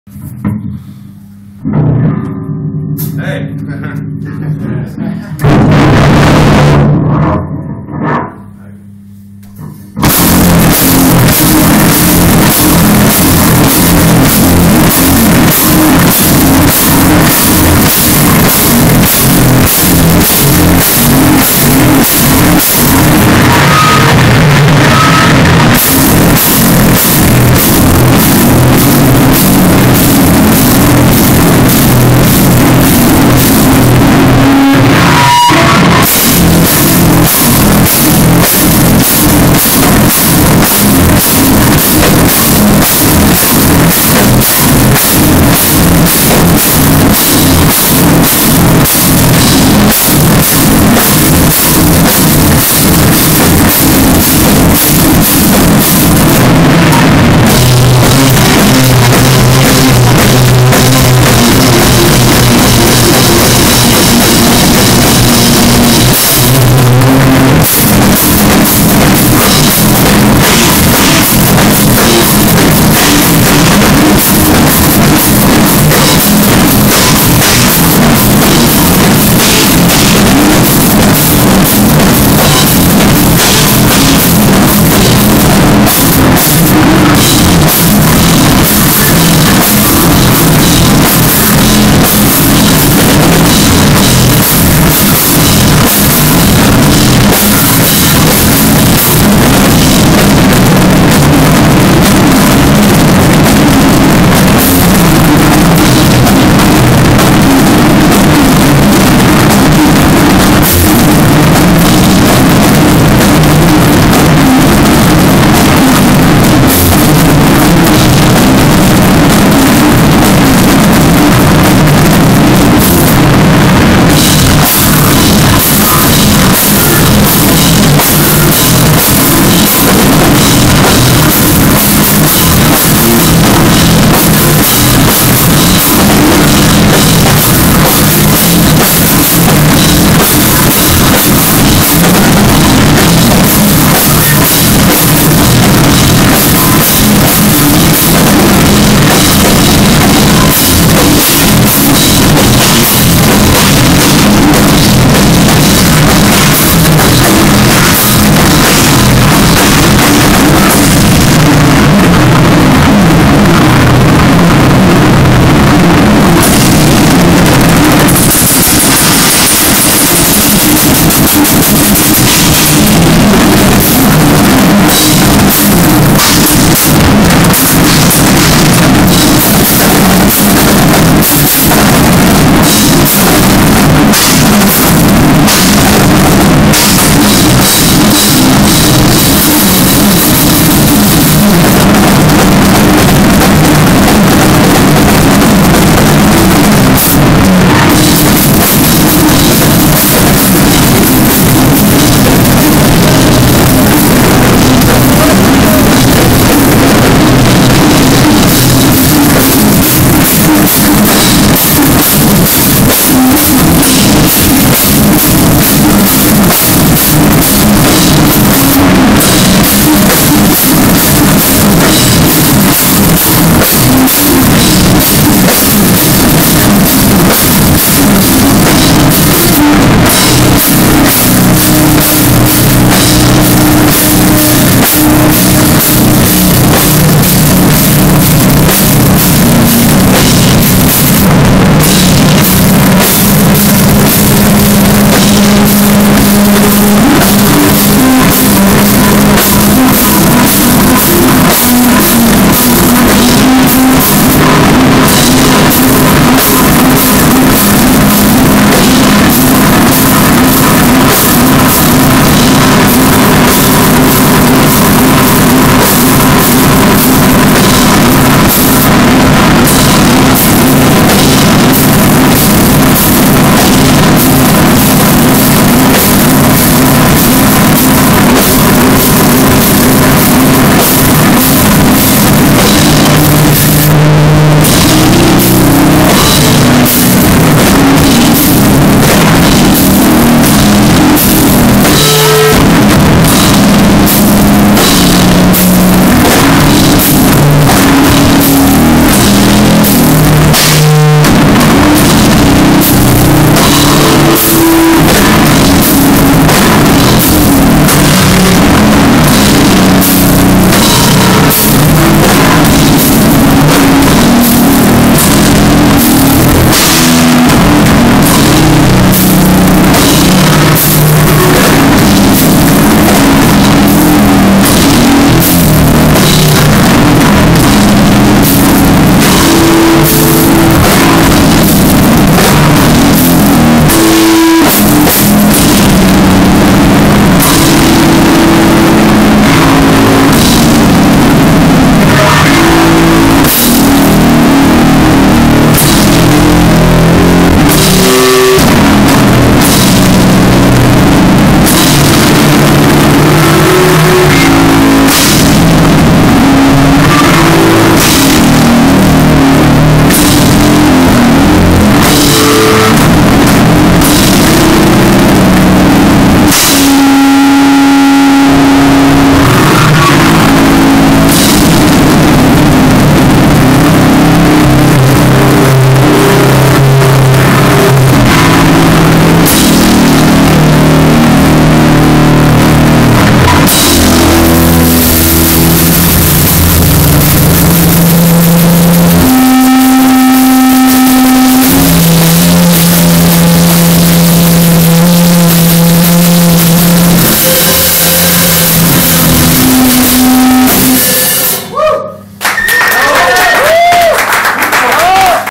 Danisco (For Beauty) @ Avv Arrebato - 29 Febrero 2012
Show en petit comité de Danisco (For Beauty) en la Avv. Arrebato